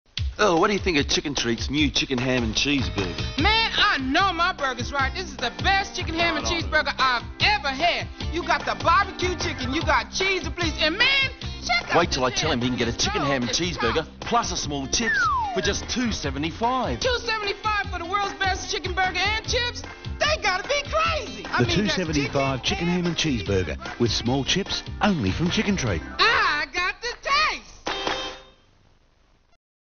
Bad Ads - Racial Stereotypes
This is an ad from 1992. I don't believe something this stereotypical ever got on television! They make the guy on the right look like bloody Jar Jar Binks... and his friend on the left acts like he's in control of the situation.